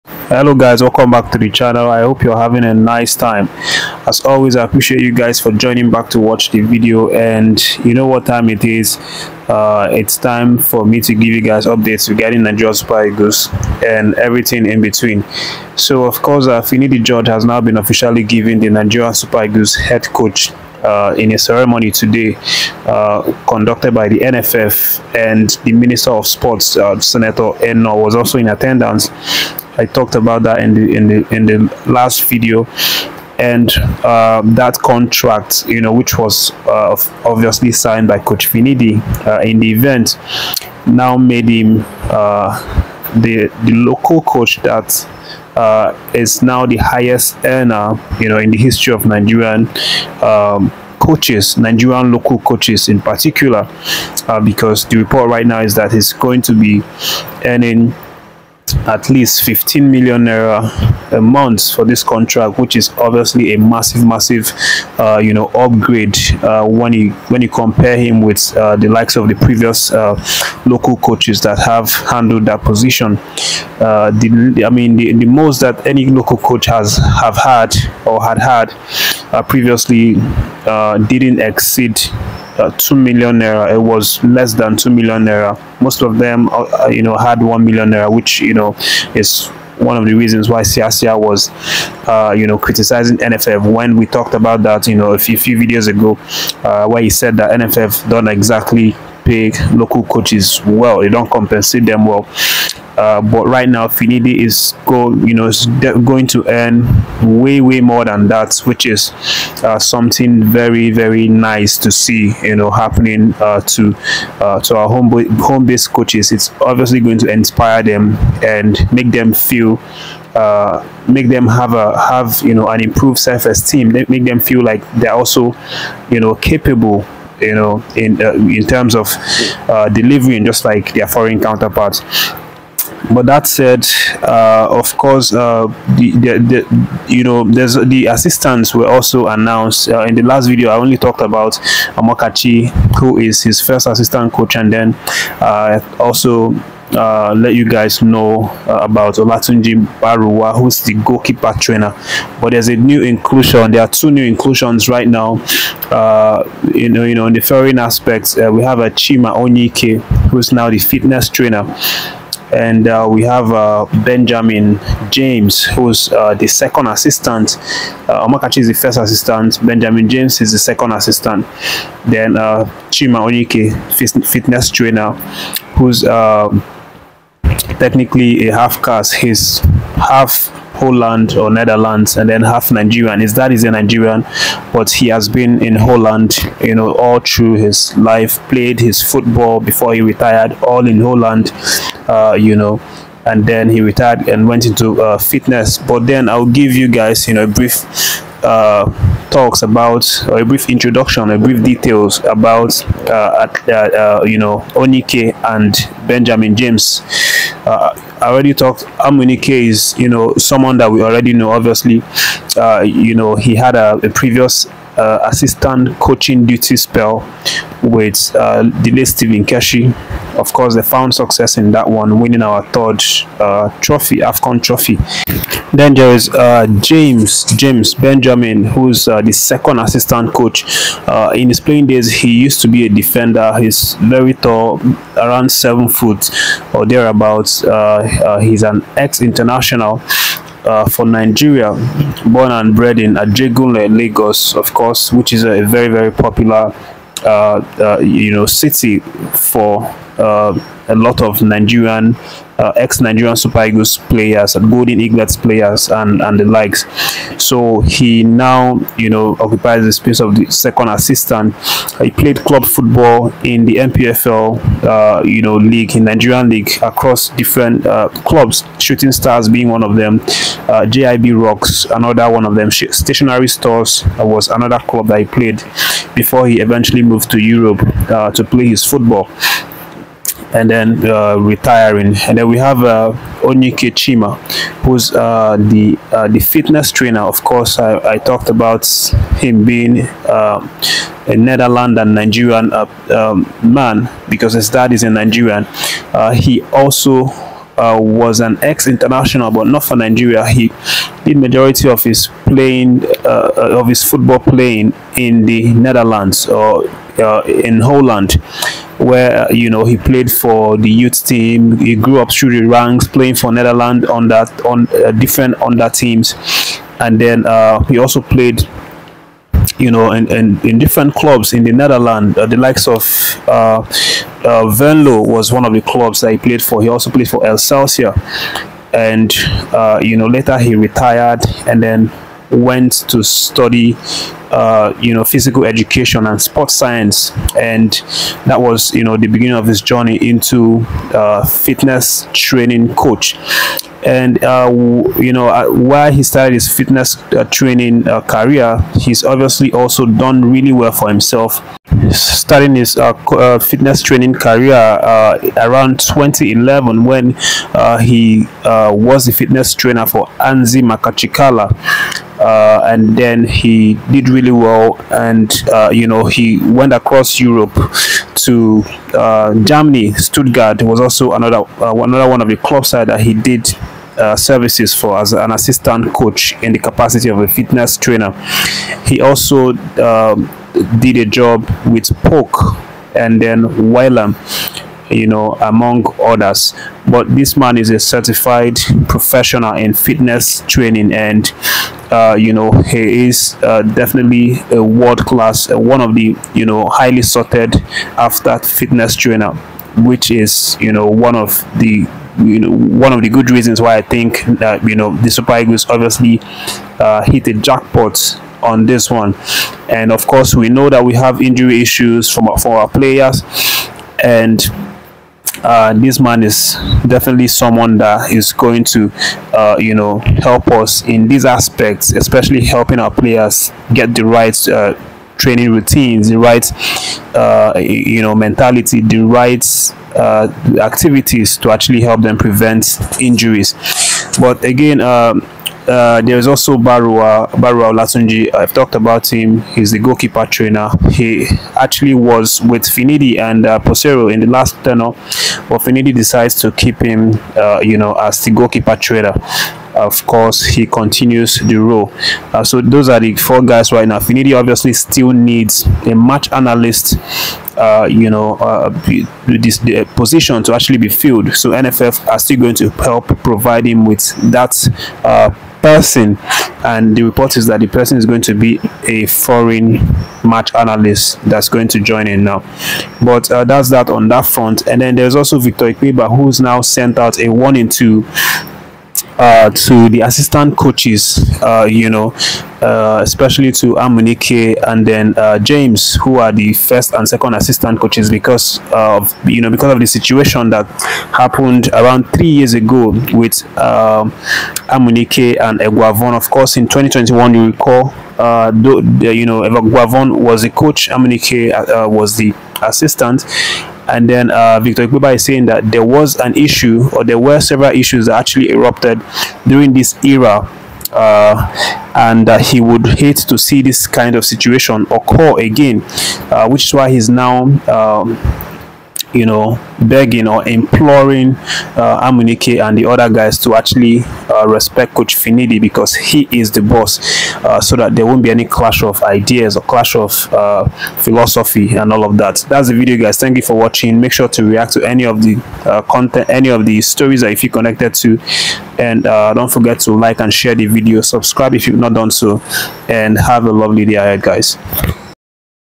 Finidi becomes highest paid Nigerian coach, two foreign appointees - James & Onyeike, Ikpeba warning
Finidi pens the most lucrative contract ever for a Nigerian coach leading the super eagles, and he gets the green light to dictate decisions for the team - a liberty we've not seen NFF extend to any local coaches before + other updates...